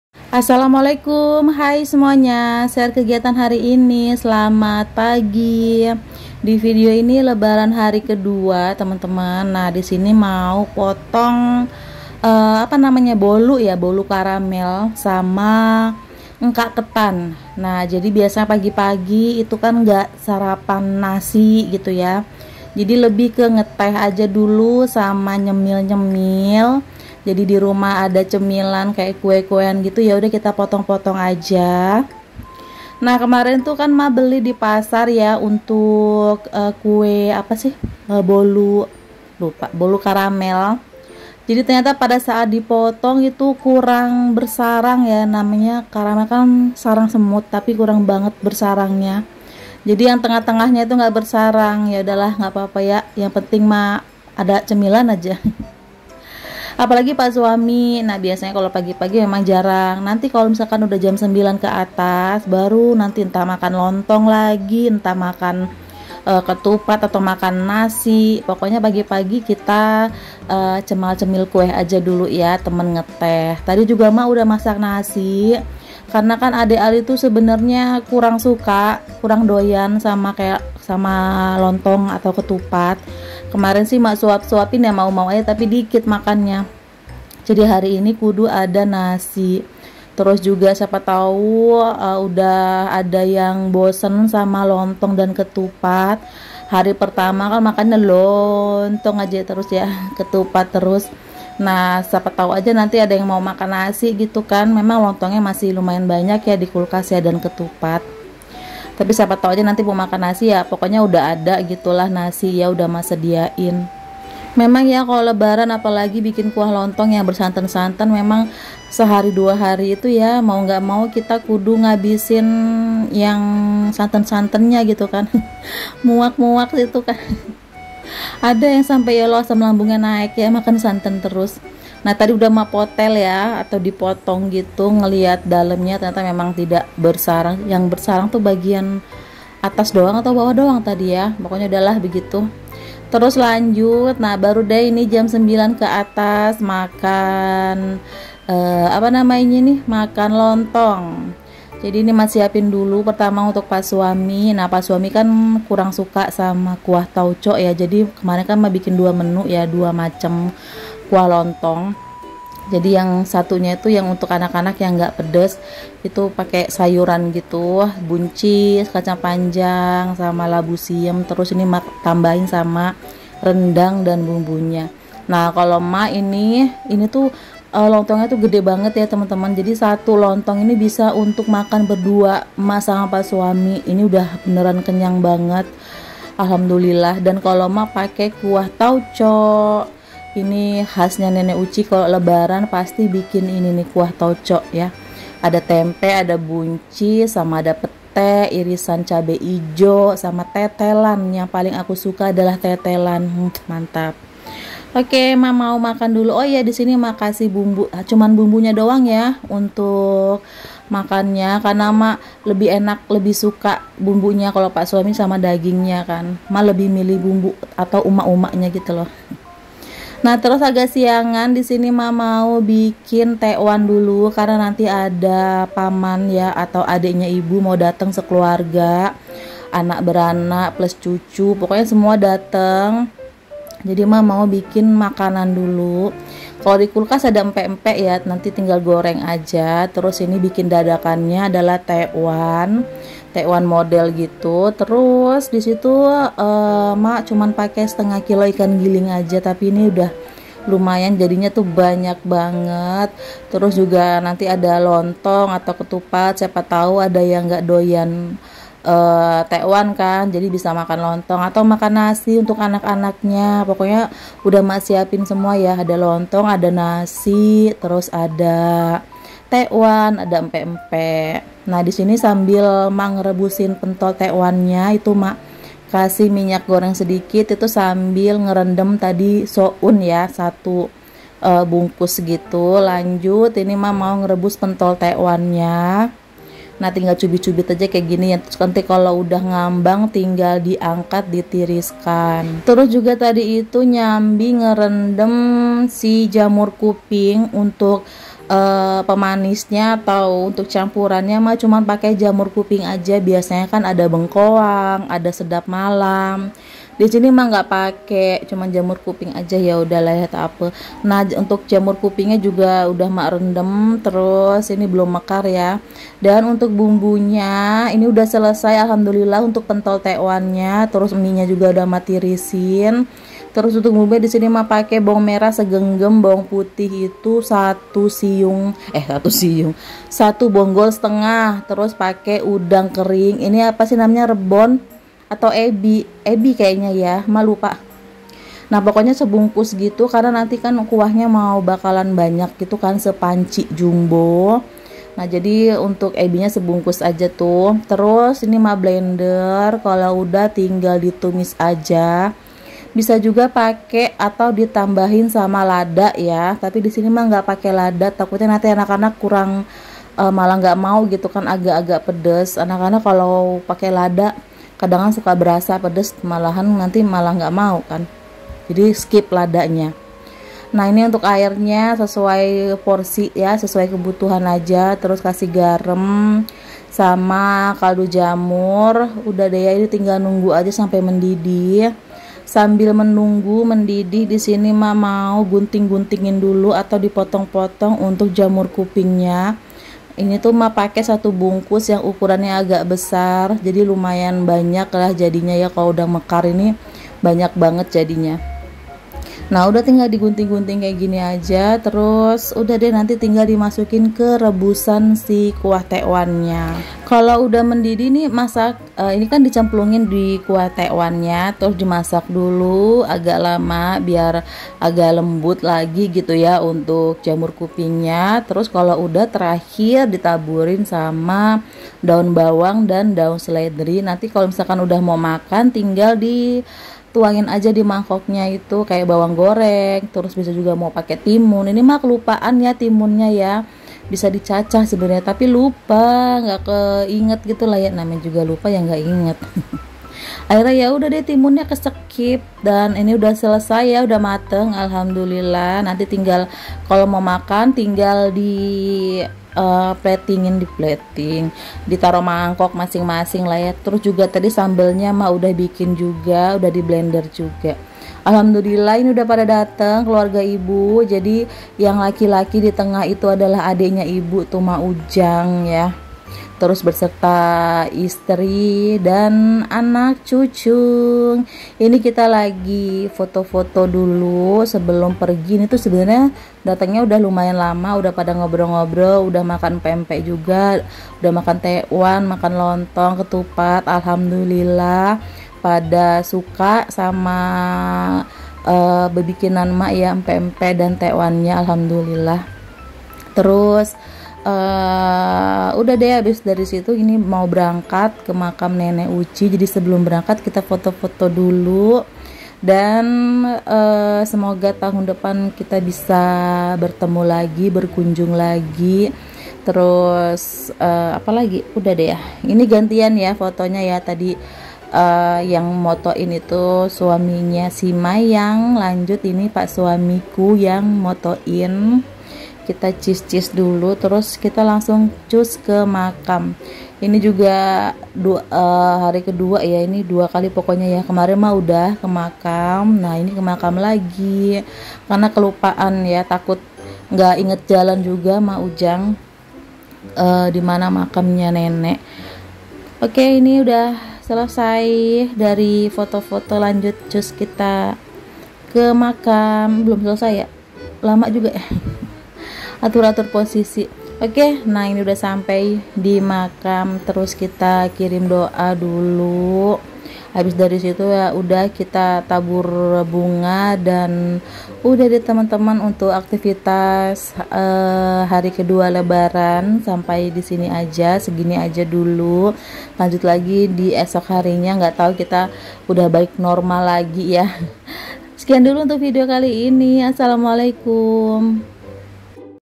KEGIATAN DI LEBARAN KE 2. BIKIN TEKWAN. SAUDARA PADA DATANG.
MAMPIR JUGA DI INSTAGRAM SAYA YAA @rumah_rifkierin

Panci Soup Mecoo
Rak bumbu 3 susun